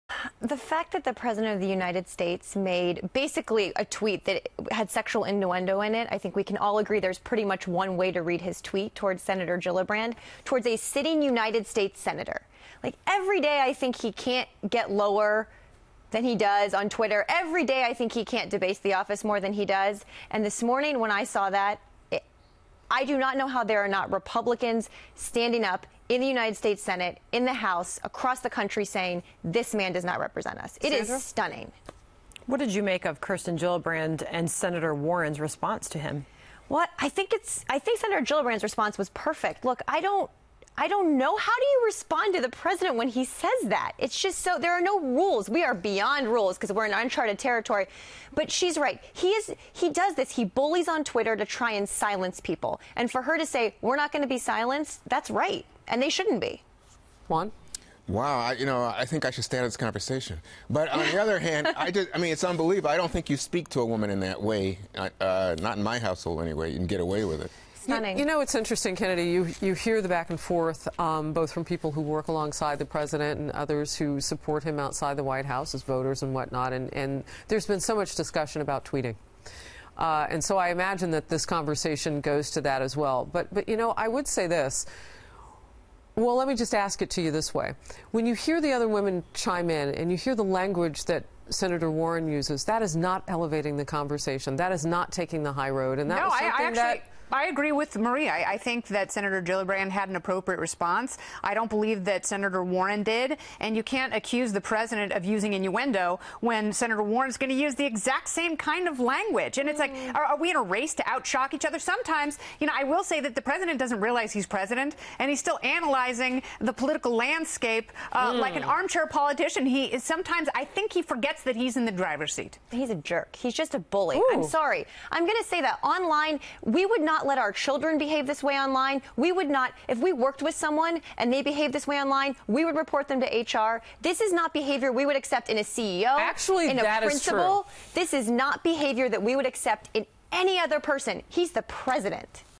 Marie Harf: President Trump is a jerk and a bully
President says spending hike is a step forward to restoring America's military might.